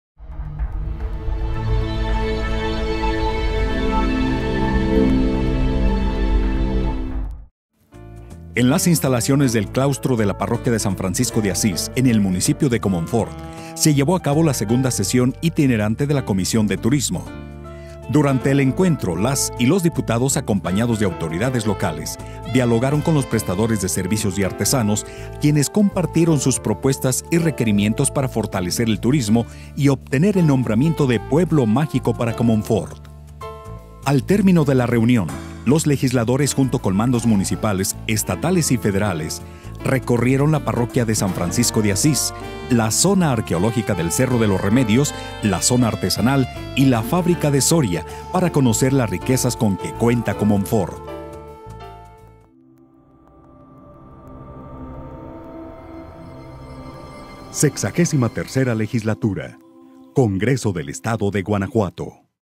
Comisión Itinerante de Turismo en Comonfort
En las instalaciones del Claustro de la Parroquia de San Francisco de Asís, en el municipio de Comonfort, se llevó a cabo la segunda sesión itinerante de la Comisión de Turismo.